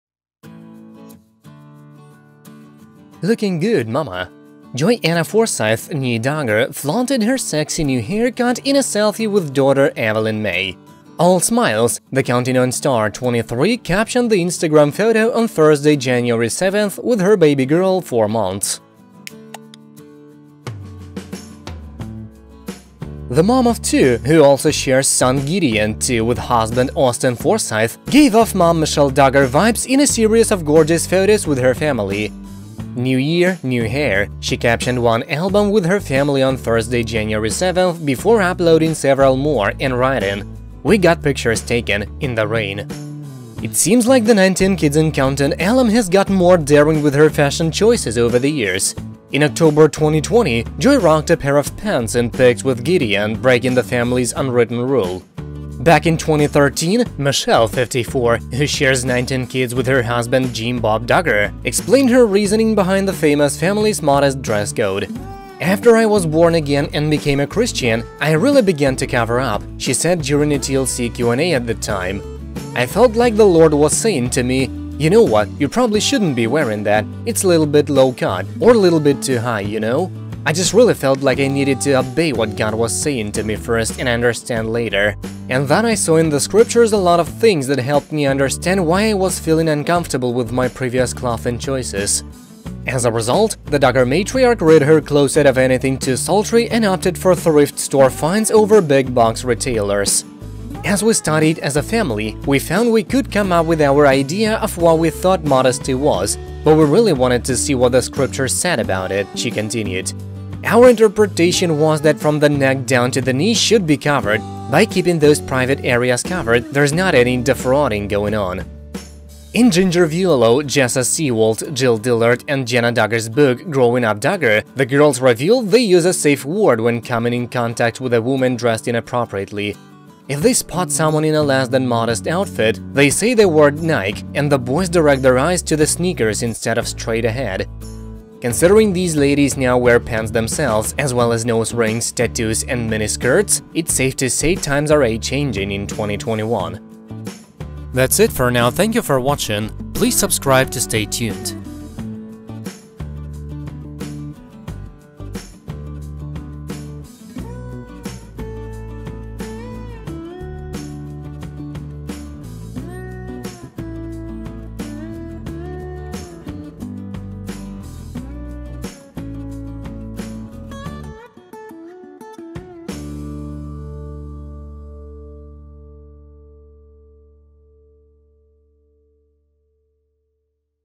Ooh, La La! Joy-Anna Duggar Flaunts Sexy New Haircut in New Photo With Daughter Evelyn: ‘All Smiles’
Lookin’ good, mama! Joy-Anna Forsyth (née Duggar) flaunted her sexy new haircut in a selfie with daughter Evelyn Mae.

“All smiles,” the Counting On star, 23, captioned the Instagram photo on Thursday, January 7, with her baby girl, 4 months.

The mom of two — who also shares son Gideon, 2, with husband Austin Forsyth — gave off mom Michelle Duggar vibes in a series of gorgeous photos with her family.

“New Year. New Hair,” she captioned one album with her family on Thursday, January 7, before uploading several more and writing, “We got pictures taken. In the rain.”

It seems like the 19 Kids and Counting alum has gotten more daring with her fashion choices over the years. In October 2020, Joy rocked a pair of pants (gasp!) in pics with Gideon, breaking the family’s unwritten rule.

Back in 2013, Michelle, 54 — who shares 19 kids with her husband, Jim Bob Duggar — explained her reasoning behind the famous family’s modest dress code.

“After I was born again and became a Christian, I really began to cover up,” she said during a TLC Q&A at the time. “I felt like the Lord was saying to me, ‘You know what, you probably shouldn’t be wearing that. It’s a little bit low cut, or a little bit too high, you know?’ I just really felt like I needed to obey what God was saying to me first and understand later. And then I saw in the scriptures a lot of things that helped me understand why I was feeling uncomfortable with my previous clothing choices.”

As a result, the Duggar matriarch rid her closet of anything too sultry and opted for thrift store finds over big-box retailers.

“As we studied as a family, we found we could come up with our idea of what we thought modesty was, but we really wanted to see what the scriptures said about it,” she continued. “Our interpretation was that from the neck down to the knee should be covered. By keeping those private areas covered, there’s not any ‘defrauding’ going on.”

In Jinger Vuolo, Jessa Seewald, Jill Dillard and Jana Duggar‘s book Growing Up Duggar, the girls revealed they use a “safe word” when coming in contact with a woman dressed inappropriately. If they spot someone in a less-than-modest outfit, they say the word “Nike” — and the boys direct their eyes to their sneakers instead of straight ahead.

Considering these ladies now wear pants themselves, as well as nose rings, tattoos and miniskirts, it’s safe to say times are a-changin’ in 2021.